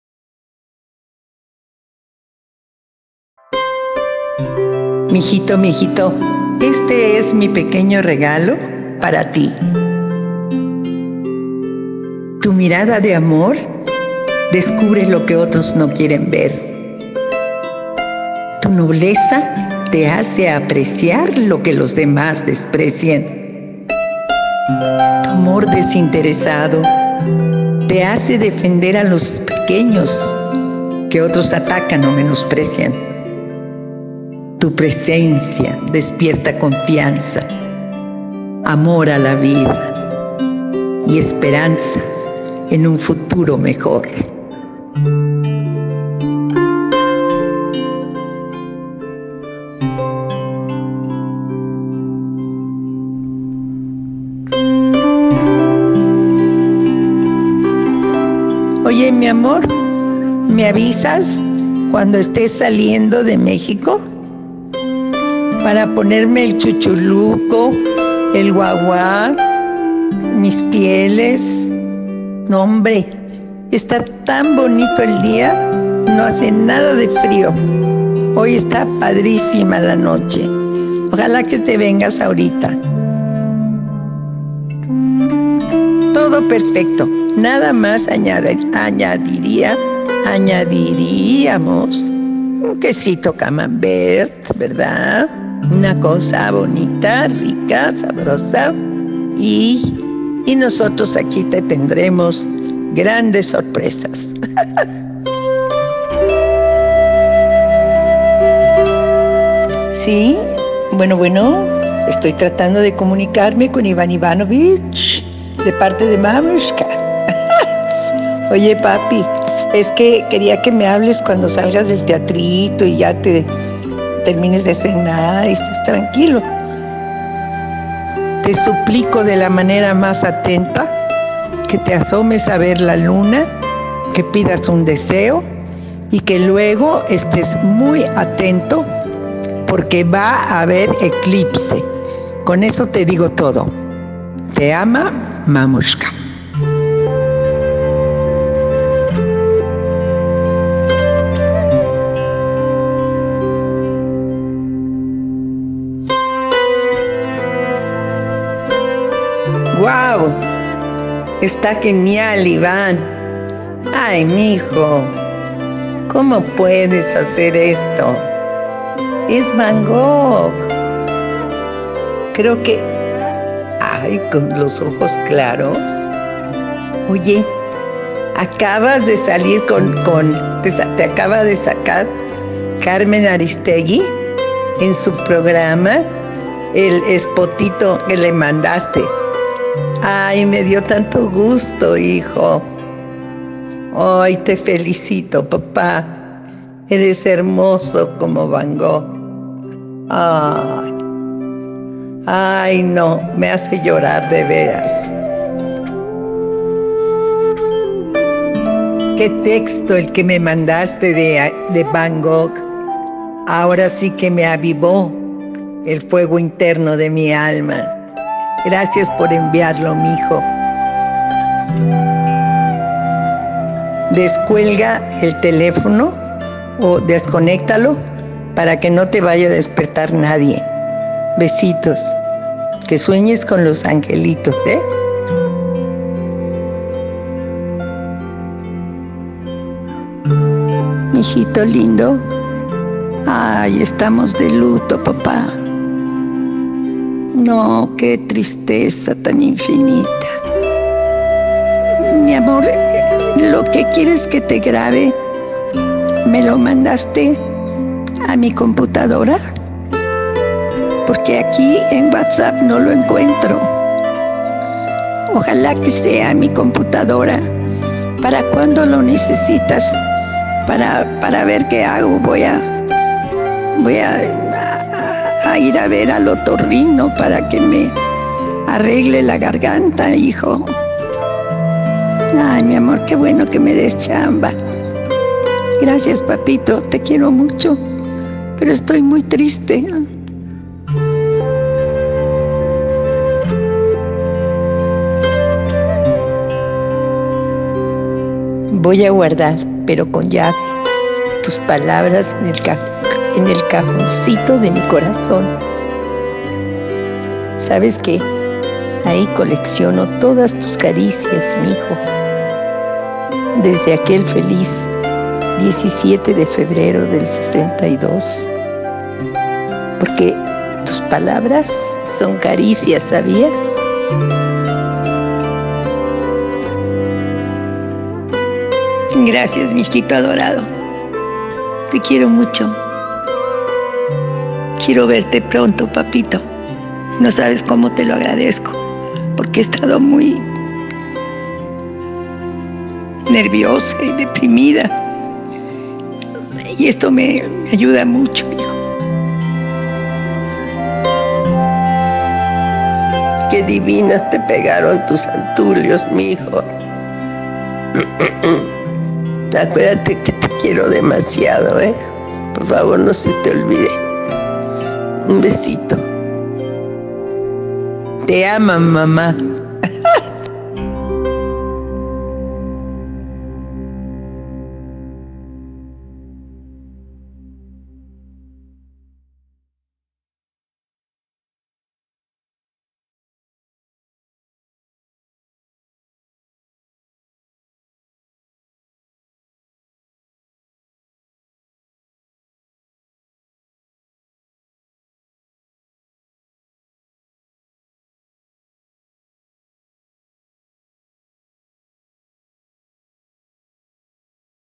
In memoriam Margarita Isabel 1941-2017 Madre de Mario Iván Martinez
Comparto este compendio de mensajes entrañables que mi madre  la actriz Margarita Isabel , fallecida el 9 de abril de 2017 me enviara por teléfono . Bendito sea el Whatsapp! pues conservaré su voz  intima para siempre.

La selección de mensajes y la musicalizacion' fueron un regalo de mi amigo Paco Aveleyra . La edición de imágenes de igual manera un presente de Ana Luna. Los abrazo agradecido maestros !

Sirva esto como un testimonio de la relación casi telepática y profundamente amorosa que gocé con quien fuera mi madre, maestra , amiga, compañera y cómplice.

Mario Iván Martinez